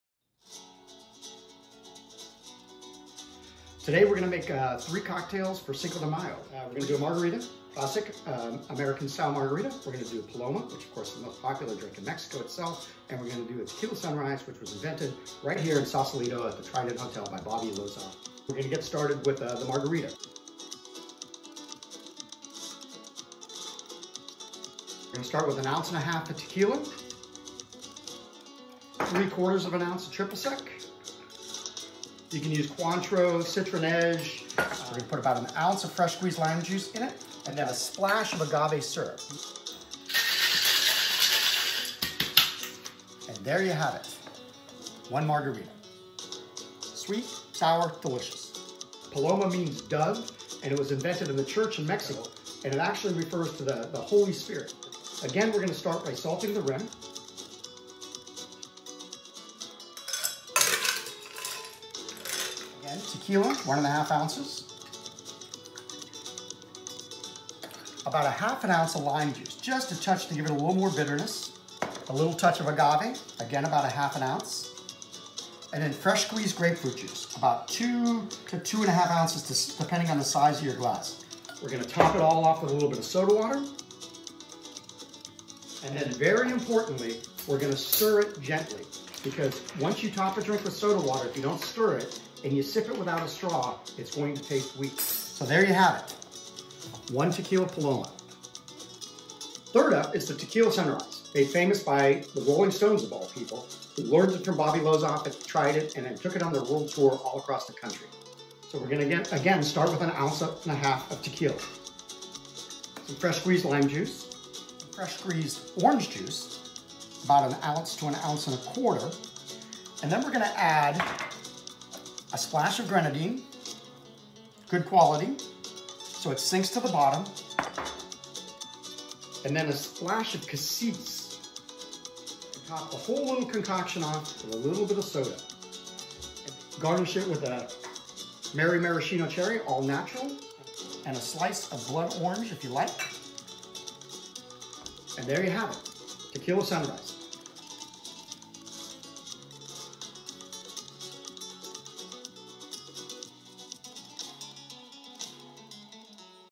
Tres Cocktails for Cinco de Mayo
Jeff Burkhart (award-winning bartender and "Barfly" columnist for the Bay Area News Group) shows us how to make a Classic Margarita, the Paloma, and the Tequila Sunrise.